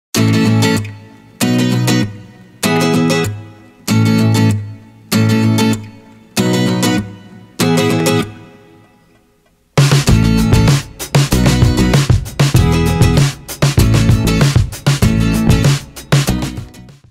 Color del mes - Septiembre 2021
¡Alegría y positividad!​
Nuestro color del mes, Novel Lilac SW 6836​
le dará ese toque dulce a tu hogar. ​